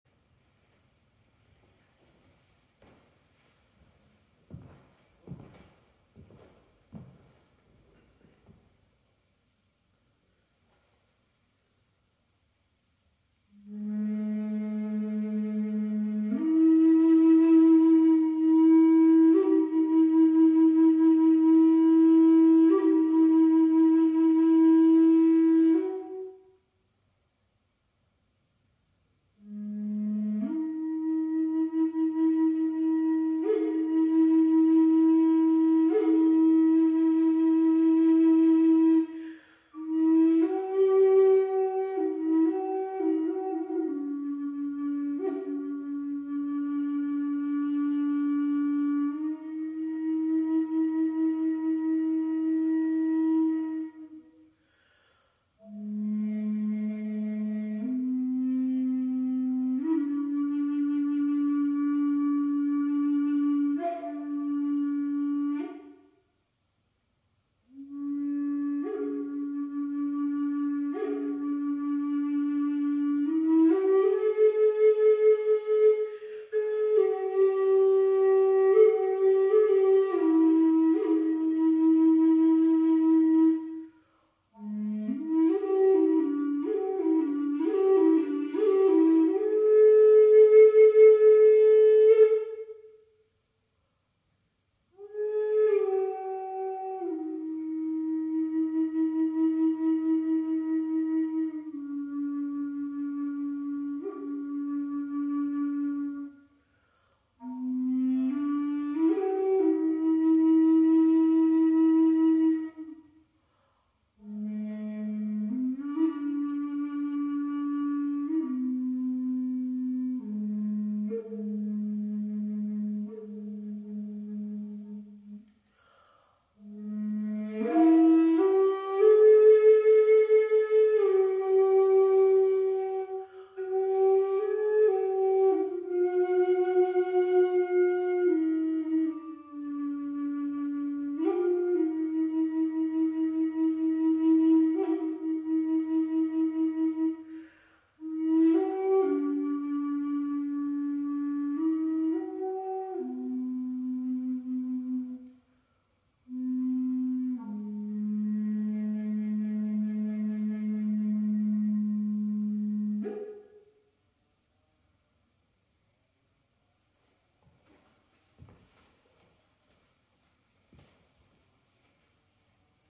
Brynn Albanese-Low Am Native Style Flute for bedside therapeutic music sessions
I am a therapeutic music practitioner and an end of life doula.
Part of my practitioner work is for the actively dying. I play low native style flutes for the slow vibrational properties of their sound production. Especially these lower ranged flutes.
Though I was a full-time concert violinist for most of my adult life, I took up the flute as a hobby about 20 years ago. I had no idea that it would turn into something that I would be playing as a service to others.
Because of the HIPAA privacy laws for patients in hospitals and skilled nursing facilities, I am not able to take video of myself playing for most of them. This is the first video that I have made thus far.
I have five flutes, one of which is a very low bass F# 432 Hz tuned side blown flute that is my actively dying go to flute and vibrates at a very low rate to help the body relax and for the organs to shut down. The violin vibrates at far too fast a rate and is two shrill for the body.
I have transitioned six people thus far in my work and I love this work. It is so sacred and such an honor to be invited into this most intimate and transformative space.